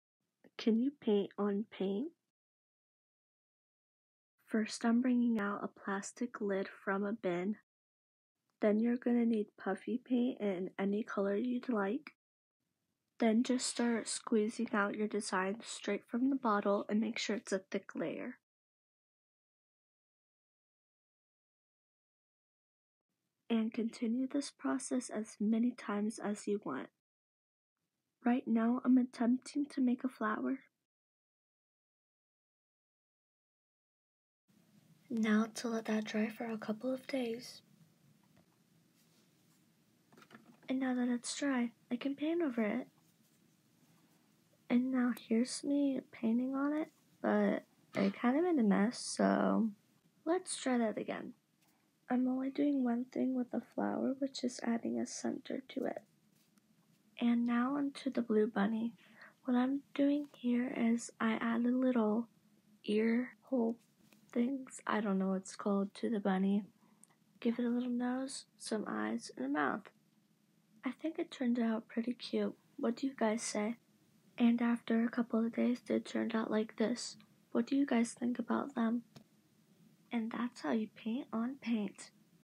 Painting on paint?! How to paint on paint 🎨
This video is of me painting on paint and showing you how to do the same

Hope you all are having an amazing day!
I do Art, collaborations, skits and more random stuff so yeah